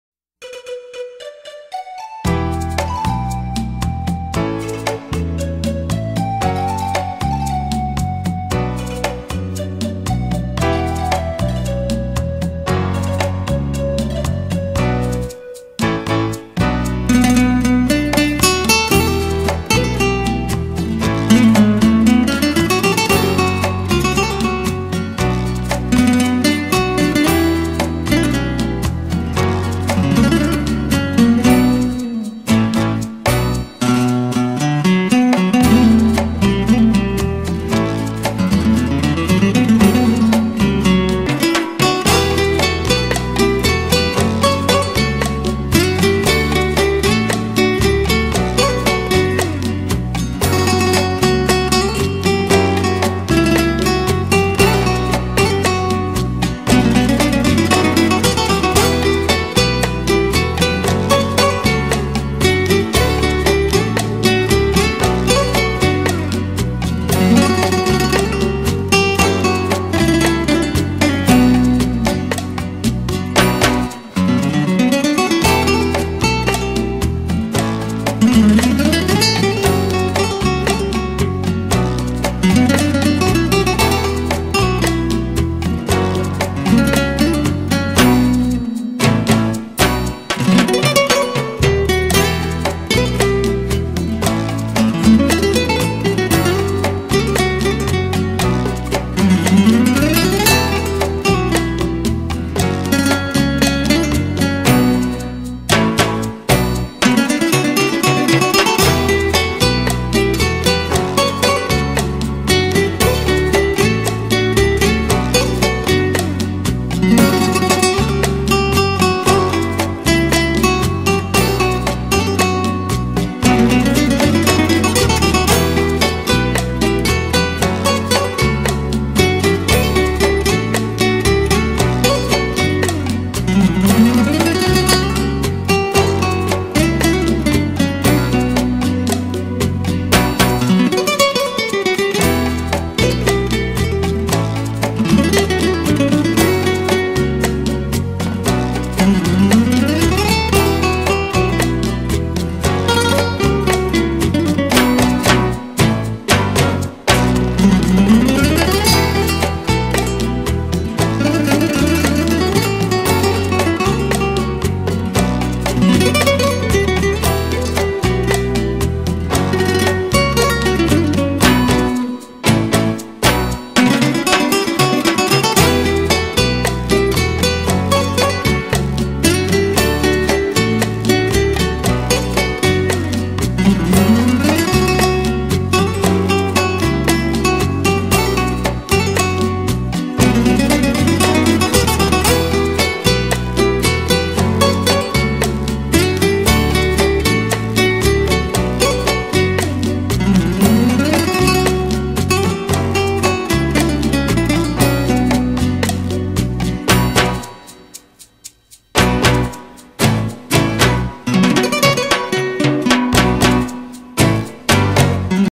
Гитара и круговорот времени
Год течет, день за днем, зима сменяет осень. Красивая музыка и работы художника на сайте